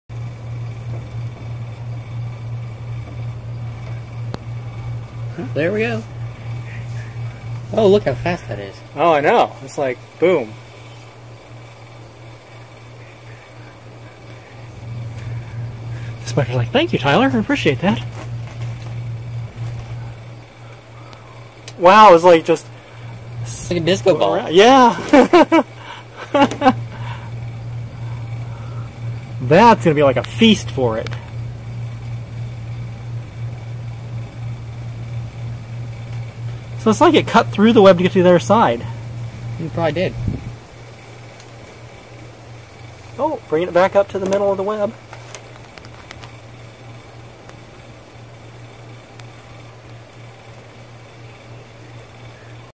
yellow garden spider wraps up grasshopper
My son fed a grasshopper to a yellow garden spider (argiope aurantia).  She left it hanging there for about 20 minutes and then we had to leave, so I don't know when/if she ate it, but it was cool to watch her wrap it up.